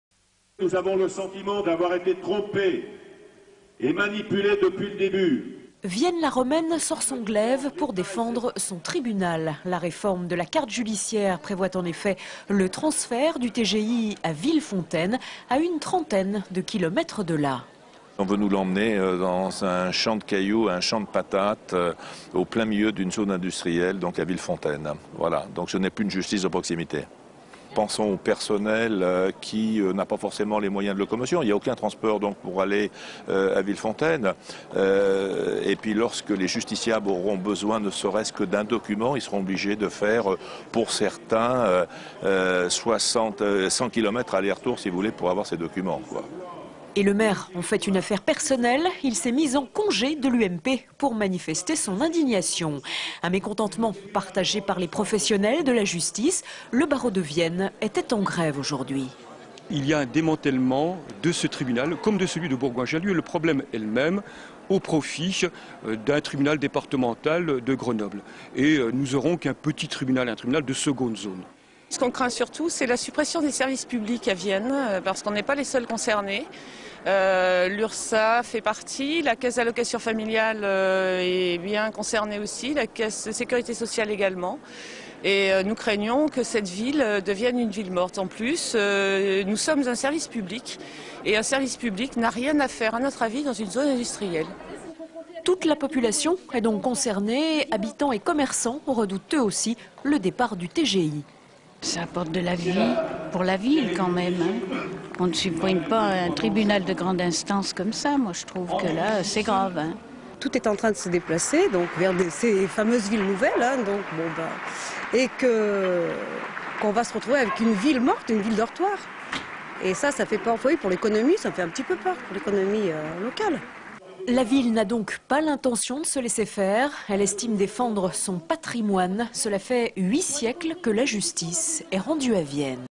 Justice : Vienne la Romaine défend son tribunal
Justice : Vienne la Romaine défend son tribunal - Lyon - Élus avocats personnels judiciaires et commerçants se sont rassemblés devant le tribunal de Grande instance pour dénoncer son prochain transfert à Villefontaine. (Pour obtenir les droits d’exploitation commerciale de cette vidéo, veuillez contacter )